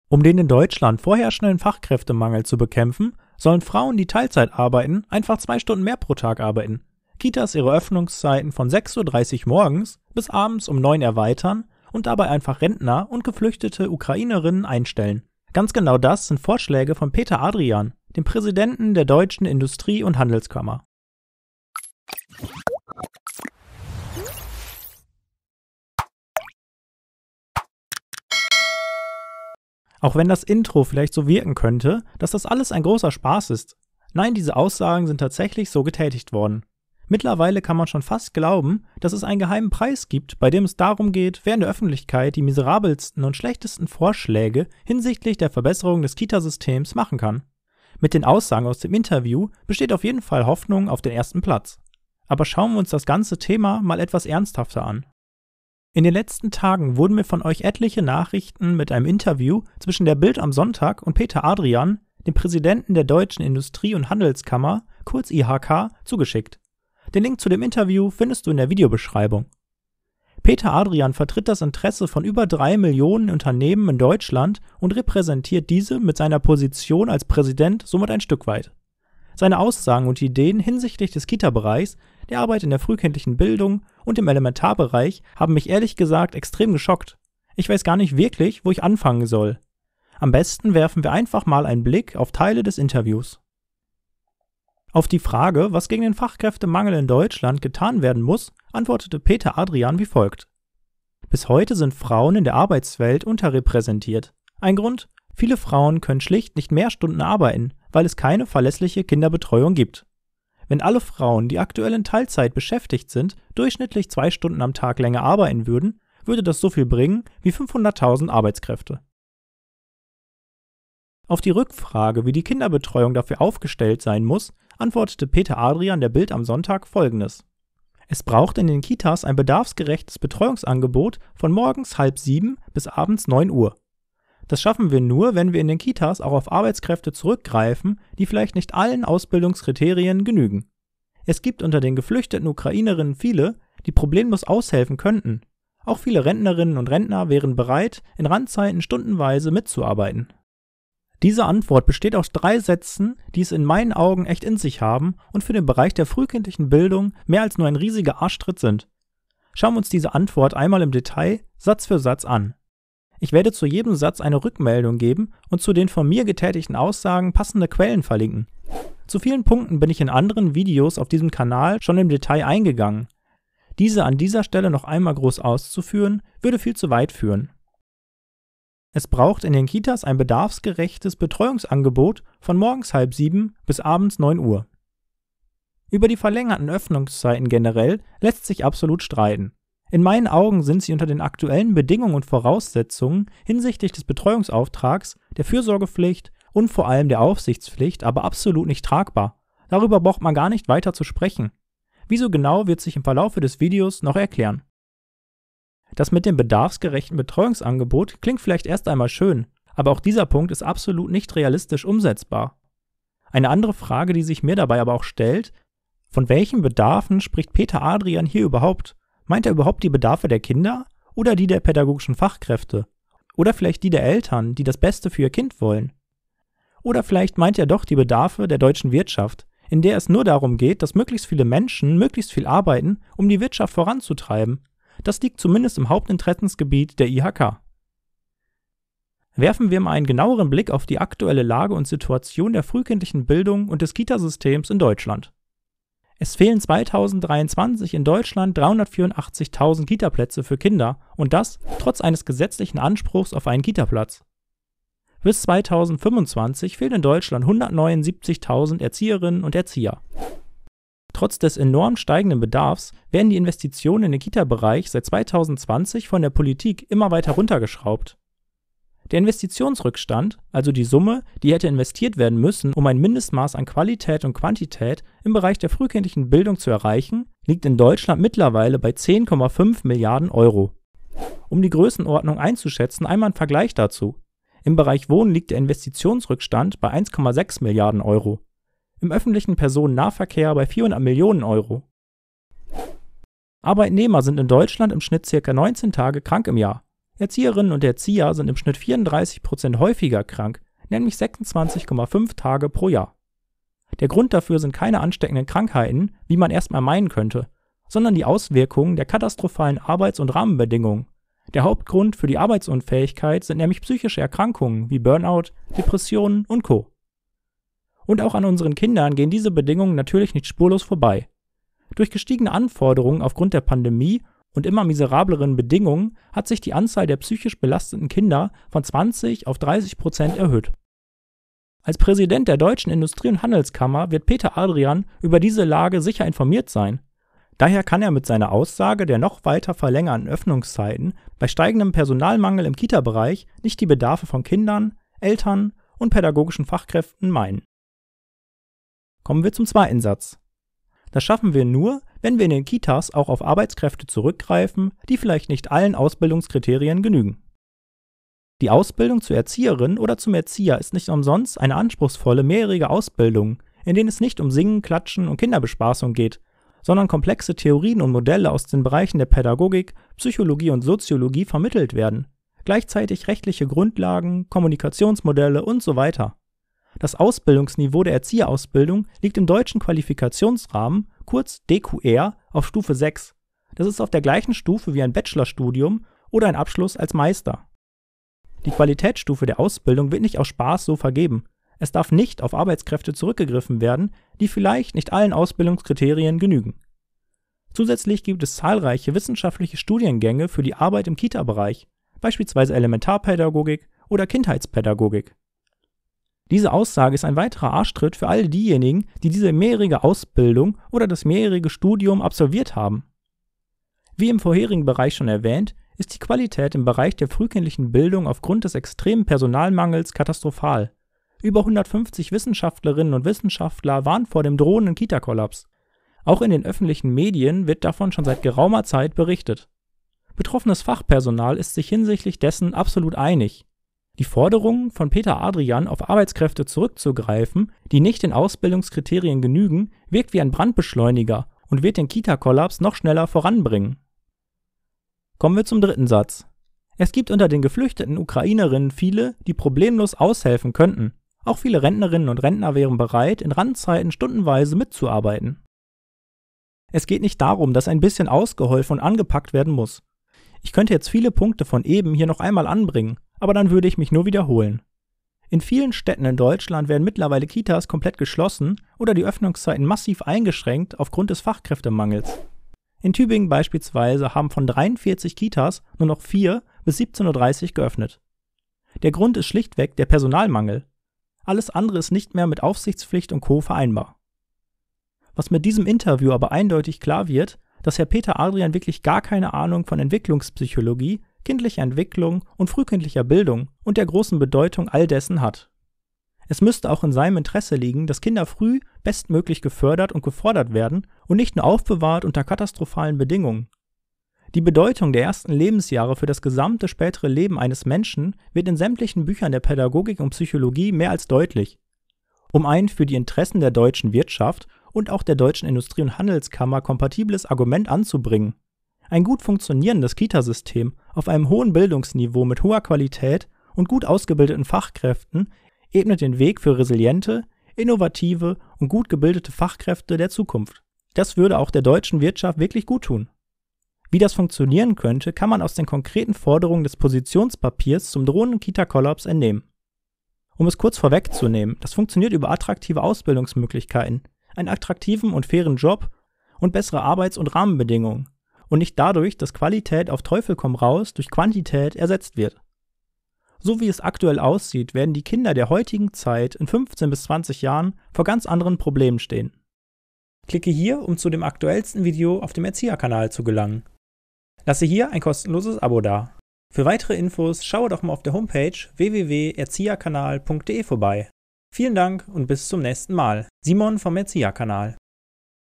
Die unglaublichen Forderungen des DIHK Präsidenten | ERZIEHERKANAL
Die Bild am Sonntag hat den Präsidenten der deutschen Industrie- und Handelskammer (Peter Adrian) zum Thema Fachkräftemangel interviewt. Dabei hat dieser unglaubliche Forderungen an das Kitasystem und die Erzieherinnen und Erzieher gestellt. Was Peter Adrian genau fordert schauen wir uns im heutigen Video genauer an.

📚 Fachwörterbuch Erzieher

📚 Fachbuch Band 1

📚 Fachbuch Band 2

📚 Fachbuch Psychologie ►►​​​​​

📚 Fachbuch Pädagogik

0:00​​​​​ - Einleitung
0:55 - Wo wurden diese Aussagen getätigt?
1:34 - Die Aussagen und Forderungen
2:58 - Kitas von 6:30 bis 21:00 Uhr öffnen
3:33 - Was ist mit "bedarfsgerecht" wohl gemeint?
3:58 - Die aktuelle Lage im Kitabereich
5:50 - Auf Arbeitskräfte zurückgreifen, die den Kriterien nicht genügen?
6:25 - Die Erzieherausbildung hat ein sehr hohes Niveau!
7:00 - Der Kita Kollaps droht
7:25 - Geflüchtete Ukrainerinnen und Rentner als Arbeitskräfte einsetzen
8:05 - Was mit diesem Interview deutlich wurde...
8:32 - Ein Argument, welches auch die deutsche Wirtschaft interessieren sollte